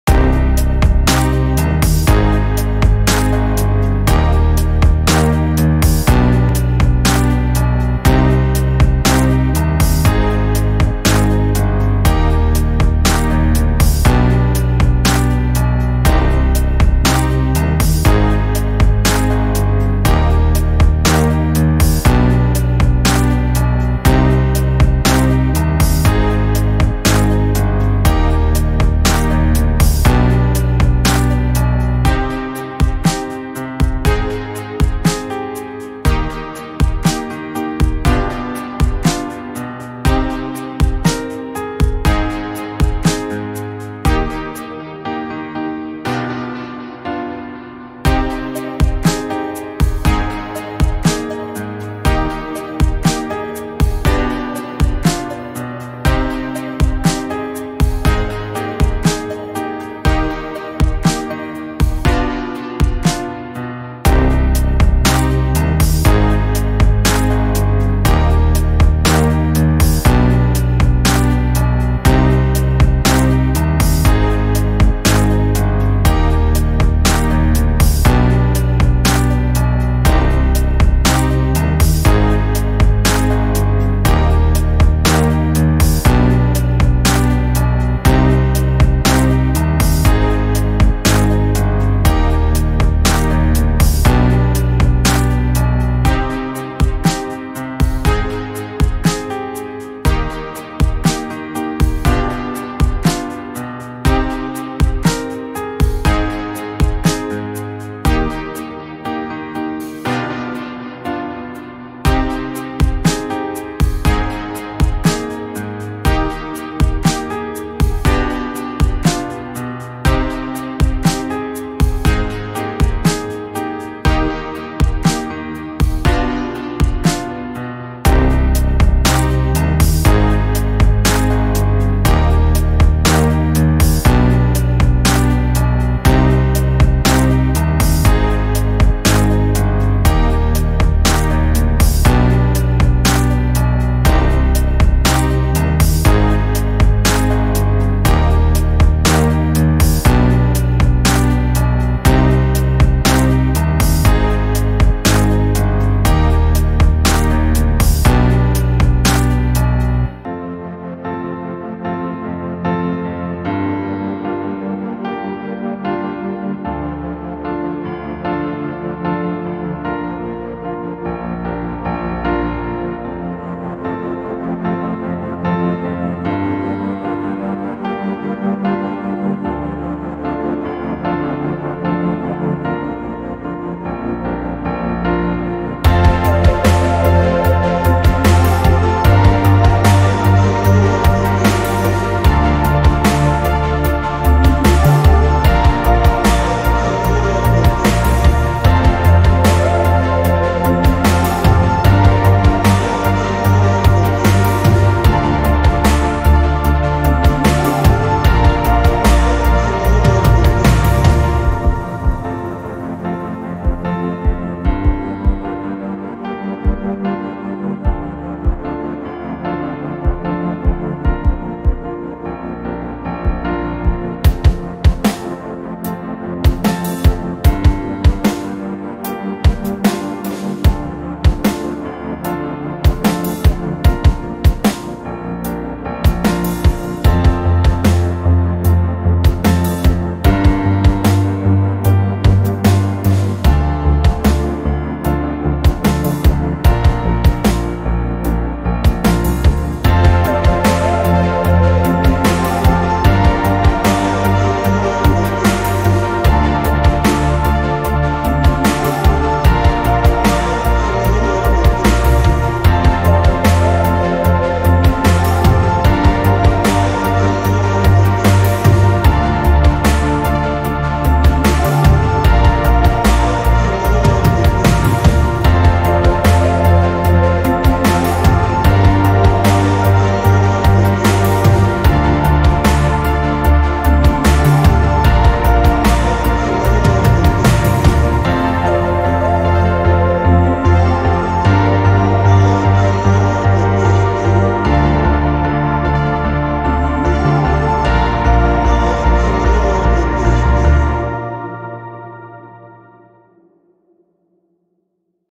The Beautiful Sounds Of Nico Staf
The beautiful Sounds of Nico Staf features with two beautifully composed songs by the artist Nico Staf. I am sure you will enjoy these beautiful pieces of music./Please comment and let me know what you think of the music.